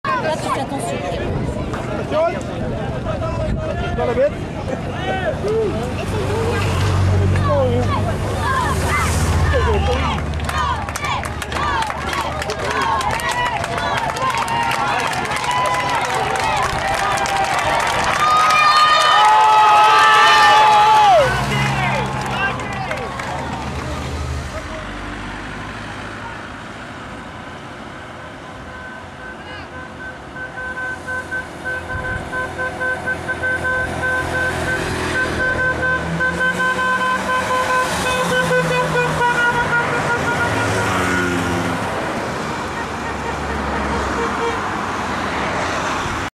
comparaison cyclisme
voici une comparaison entre le tour de guyane et celui de france